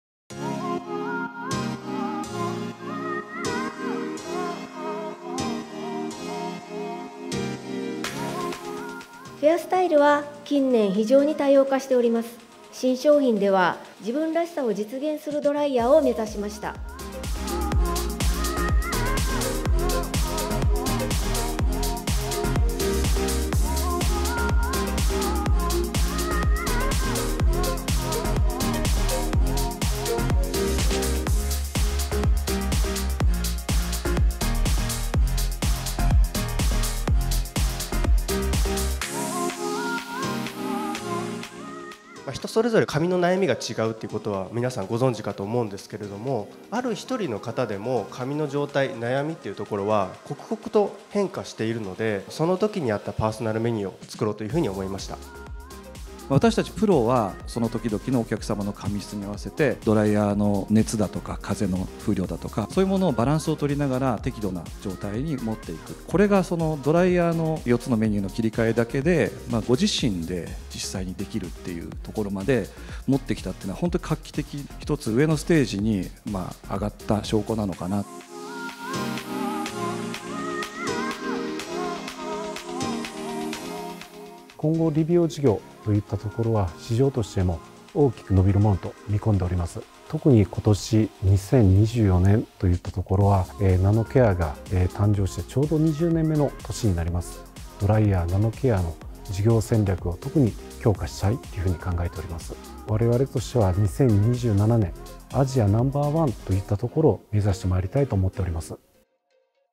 「ヘアードライヤー ナノケア」新ライン発表会 ダイジェスト PR動画【パナソニック公式】
2024年7月23日　「ヘアードライヤー ナノケア」新ライン発表会　ダイジェスト　 PR動画

・ヘアードライヤー ナノケア 「nanocare　ULTIMATE」 EH-NC80

・ヘアードライヤー ナノケア 「nanocare　ULTIMATE」  EH-NC50